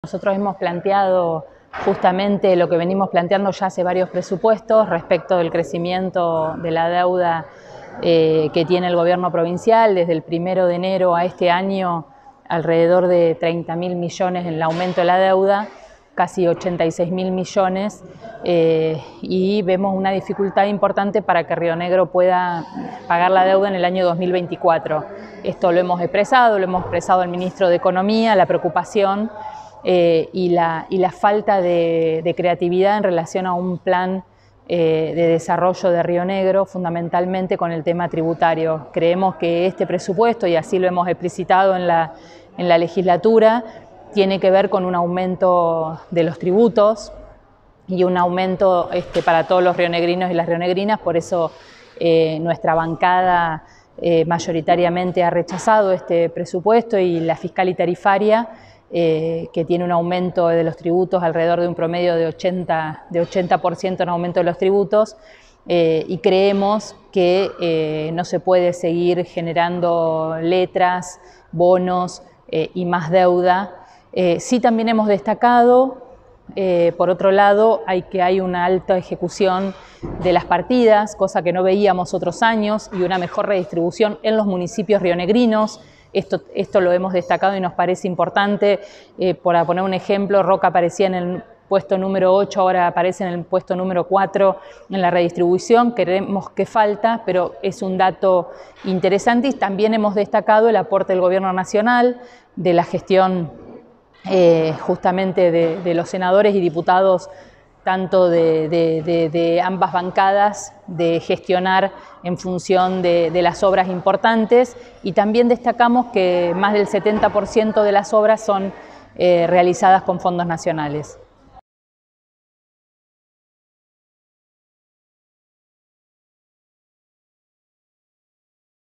Leg. María Eugenia Martini: la Legislatura aprobó el Presupuesto 2023
La Legislatura de Río Negro aprobó esta tarde el Presupuesto General de Gastos y Cálculo de Recursos para la Administración Pública Provincial, correspondiente al Ejercicio Fiscal 2023. La ley fue sancionada por mayoría, con 34 votos a favor y 9 en contra, con la oposición de Juntos por el Cambio y la mayoría del bloque del Frente de Todos.
La mayoría del bloque del Frente de Todos votó en forma negativa. María Eugenia Martini y Marcelo Mango criticaron el endeudamiento, que la deuda pública provincial siga aumentando, y las sub ejecuciones del período actual. Criticaron que en el Presupuesto 2023 no hay una propuesta política de cambio, al seguir los mismos parámetros de los anteriores. Martini cuestionó el incremento tributario que prevé el Presupuesto. Mango pidió fortalecer el Presupuesto y la inversión en políticas de Salud, Educación y Alimentación “para que con Democracia se coma mejor, se tenga mejor salud y se pueda educar más a las rionegrinos y los rionegrinos”.